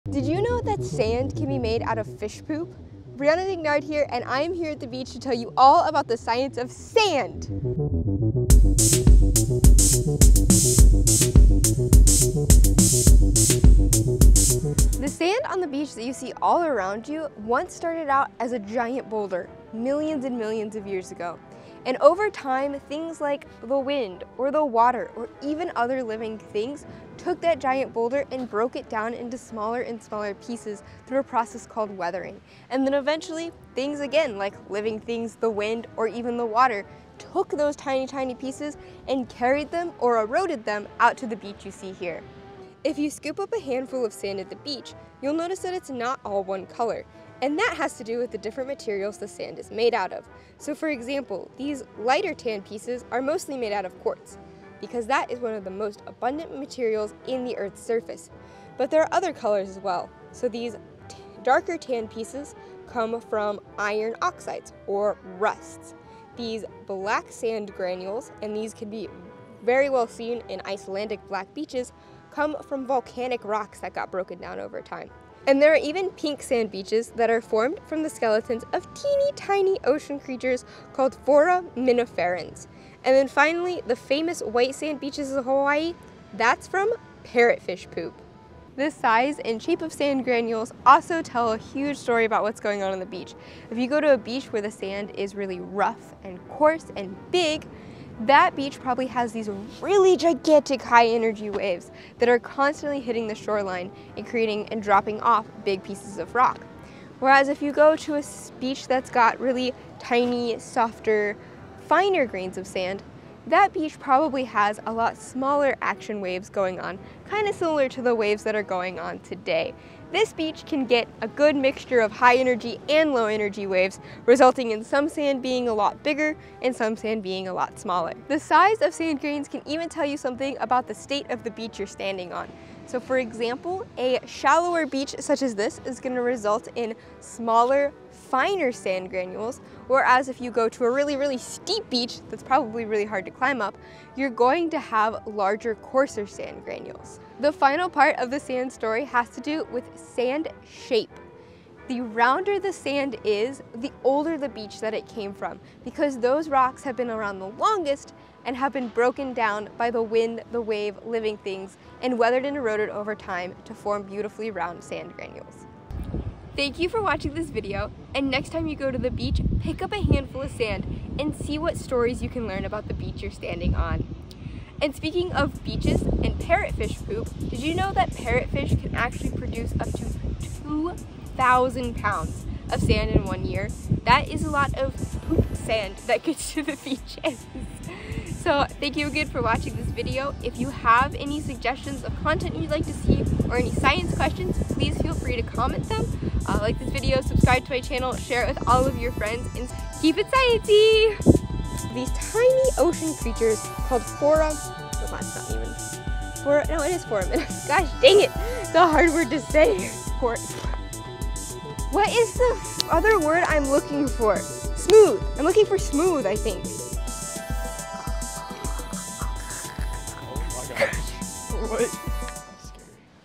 The beaches are made of poop! | How Sand Is Formed
I'm ending the summer of field trips and travel with another trip back to the beach! Today we're learning all about sand! Sand tells the story of every beach and you would be surprised how much you can learn just from picking up a handful.